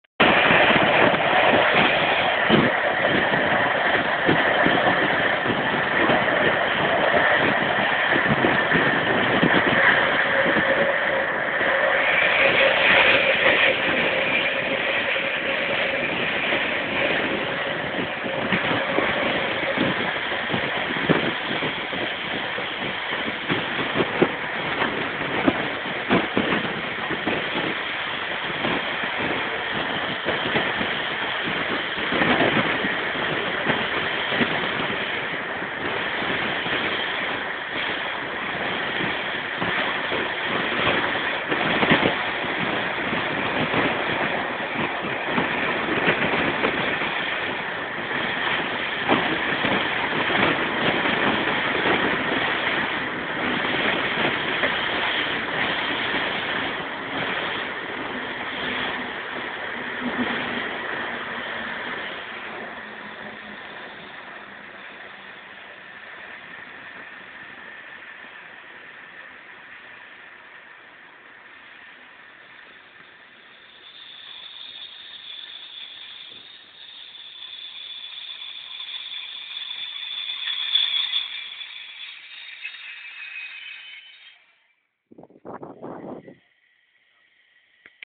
řekl jsem 120 aneb ZSSK 263.010
jízda na ZSSK 263.010 kolem 120km/h na trati směr Bratislava.